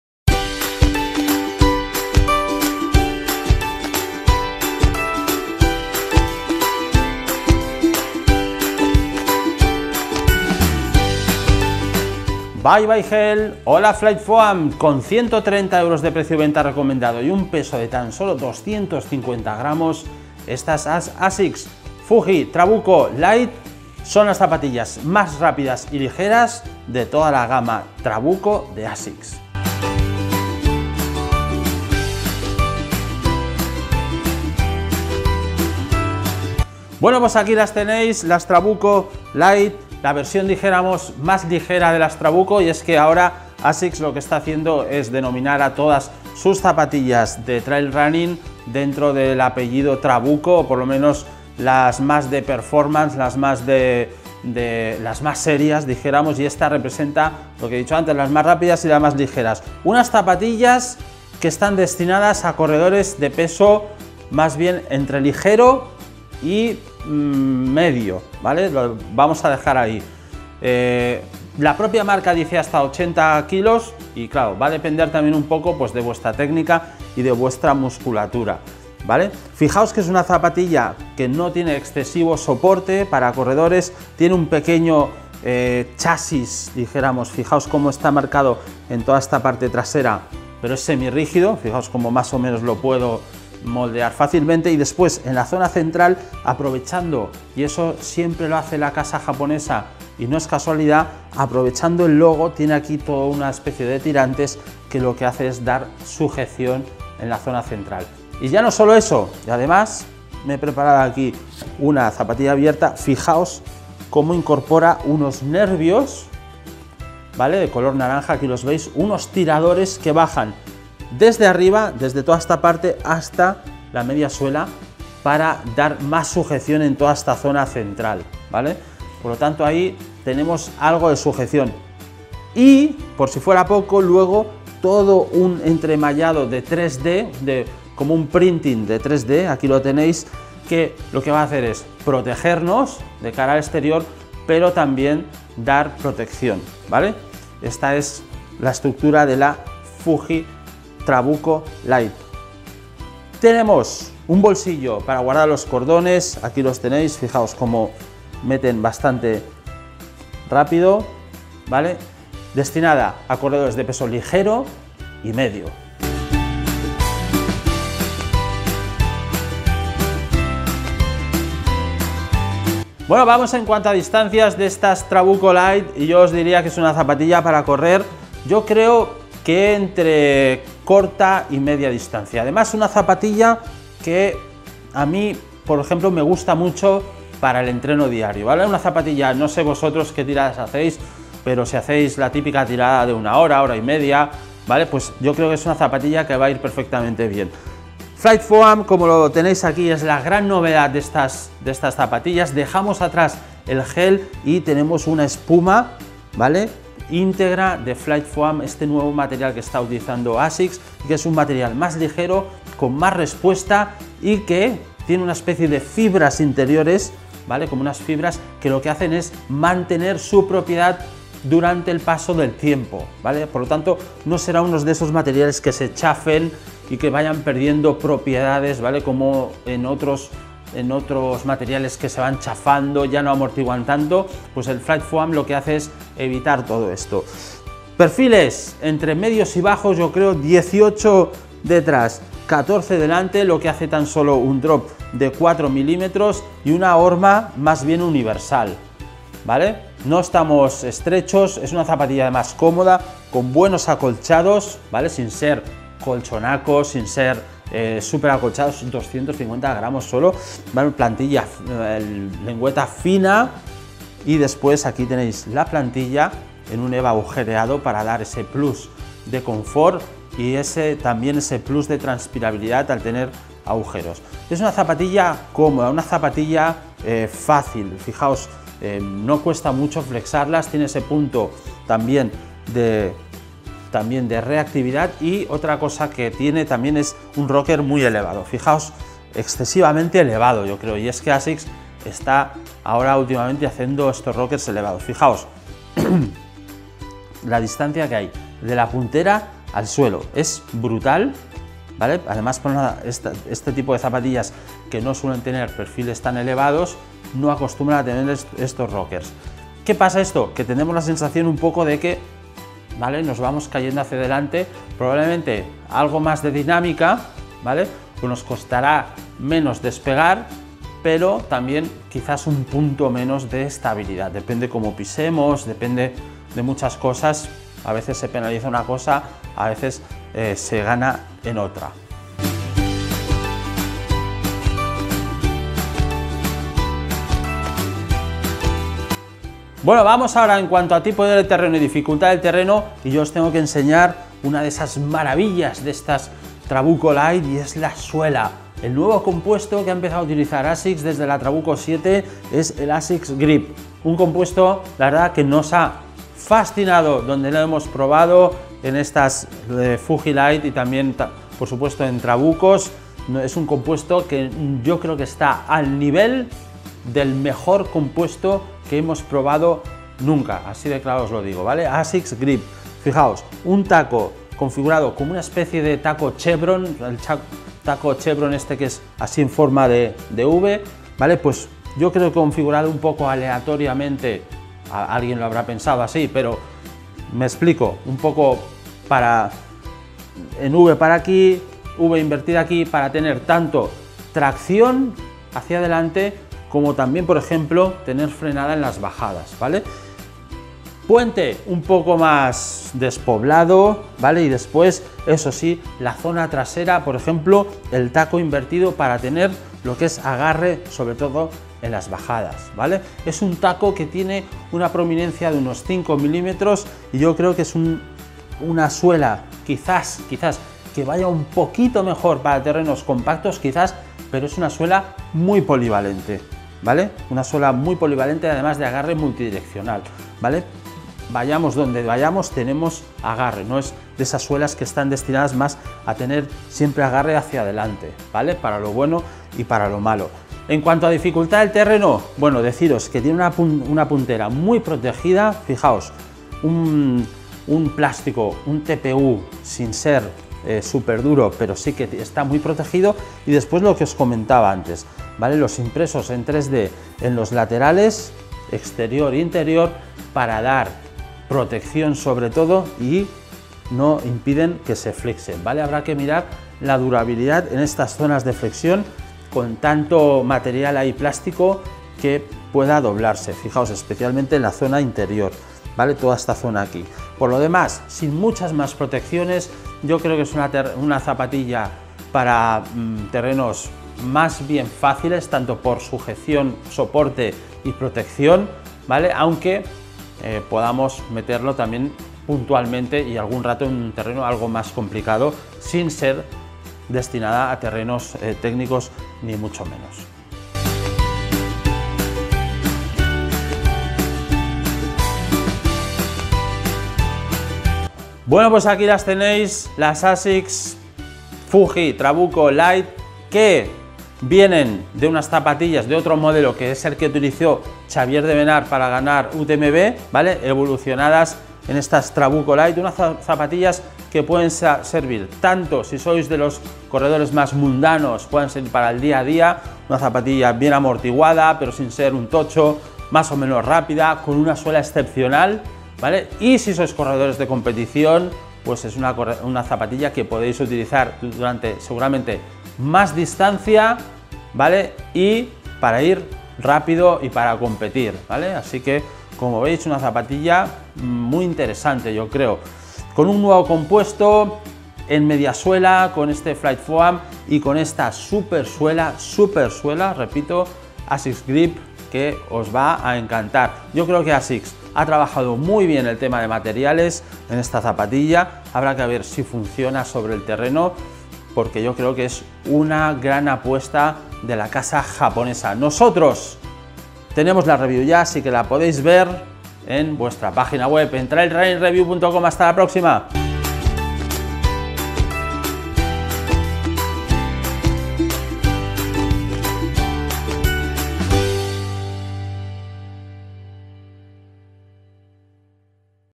Asics FujiTrabuco Lyte Review - La hermana pequeña de las FujiTrabuco
Asics FujiTrabuco Lyte Review

Las  Asics FujiTrabuco Lyte son las nuevas zapatillas de la casa japonesa de la familia FujiTrabuco. Unas zapatillas que podríamos situar por debajo de las clásicas Asics FujiTrabuco, más ligeras, menos estructuradas y protegidas con lo que ganaremos en dinamismo y velocidad.

La suela de las Asics FujiTrabuco Lyte está formada por el compuesto AsicsGrip, un material de la propia casa que ya hemos visto en los últimos modelos de Asics. Probando otros modelos con este compuesto hemos quedado encantados con el excelente agarre y se sitúa fácilmente al mismo nivel que los mejores materiales del mercado. Contiene un taqueado agresivo con forma de flecha multidireccional que creemos que le dará mucha polivalencia a la zapatilla en distintos tipos de terreno.

En la media suela, las Asics FujiTrabuco Lyte incorporan el FlyteFoam y dejan de lado el compuesto Gel que ha acompañado durante muchos modelos las media suelas de Asics. Con perfiles medios que buscando un buen equilibrio entre amortiguación y dinamismo.

El upper de las Asics FujiTrabuco Lyte es de un tejido bastante flexible. Para darle un poco más de estructura incorpora unas impresiones 3D que se extienden por los dos lados, haciéndose más grandes en la zona del talón. Sin ser una zapatilla excesivamente protegida, sí que incorpora un termosellado de plástico en la puntera para evitar dañarnos con las piedras.

Con las Asics FujiTrabuco Lyte encontraremos unas zapatillas menos pesadas para corredores que las FujiTrabuco clásicas les parecían demasiado toscas.

1- 0:30 - Tipo corredor.
2- 2:47 - Distancia.
3- 5:36 - Tipo y dificultad del terreno.
4- 8:25 - Conclusiones.

Imágenes en movimiento grabadas con la cámara Insta360 ONE X

TRAILRUNNINGReview